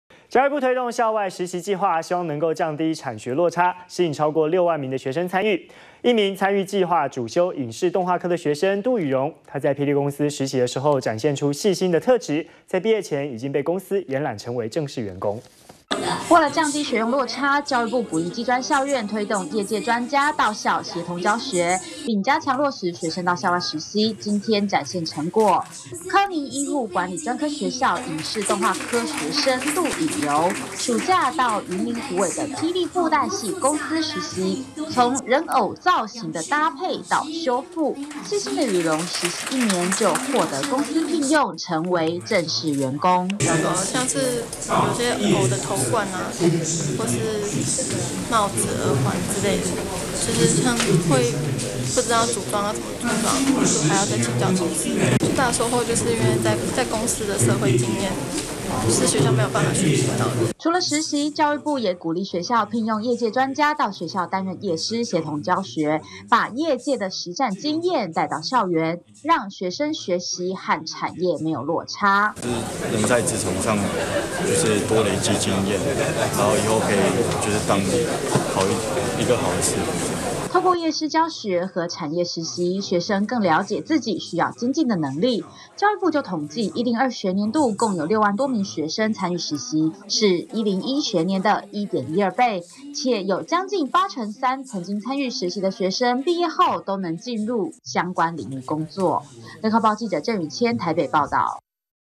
【2014.06.17】專校生"霹靂"實習 獲聘正式員工 -udn tv
教育部推動校外實習計畫，希望降低產學落差，吸引超過6萬名學生參與。一名參與計畫、主修影視動畫科的學生杜雨容，在霹靂公司實習的時候，展現細心的特質，畢業前已經被公司延攬成為正式員工。完整內容 , FB , Twitter , G+